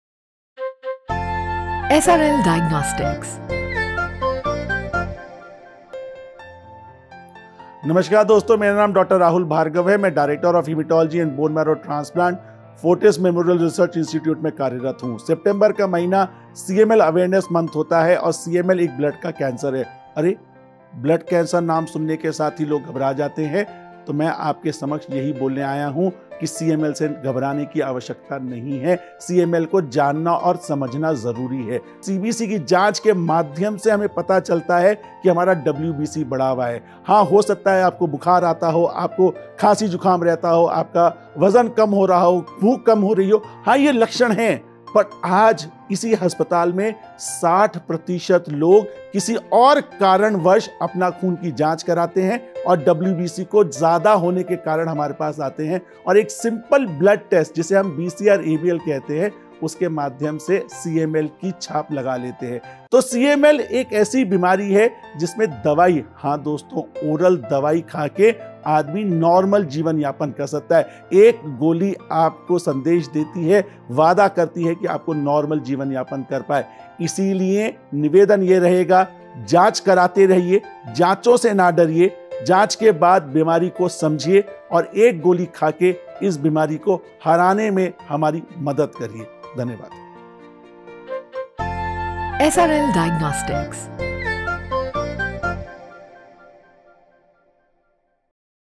Chronic Myeloid Leukemia (CML) and its diagnosis
Dr. Rahul Bhargava, Principal Director, Hematology, Hemato-oncology & BMT FMRI, Gurugram, talks about Chronic Myeloid Leukemia (CML)—a blood cancer that affects the white blood cells and tends to progress slowly over many years and how simple blood tests like CBC and BCR-ABL Quantitative IS can help in diagnosing the condition early.

This CML Awareness Month, get yourself tested with SRL Diagnostics if you're seeing symptoms of the disease.